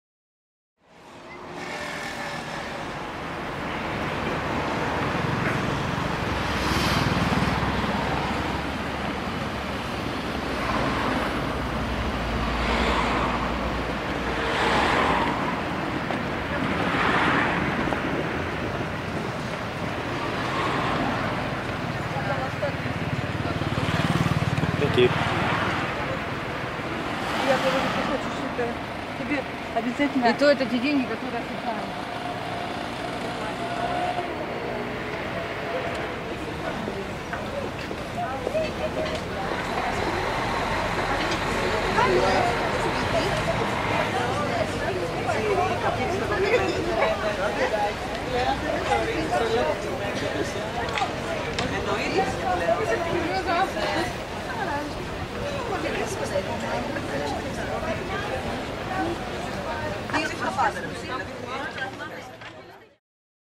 Crete - Heraklion - 7 May 2013
Heraklion, also known as Iraklio, is a port city and the capital of the Greek island of Crete. It’s known for the Palace of Knossos, just outside the city. The huge archaeological site dates back thousands of years to the Minoan civilization, and includes frescoes and baths. Guarding the city’s Venetian port is the 16th-century Koules fortress. Heraklion Archaeological Museum has a large collection of Minoan art.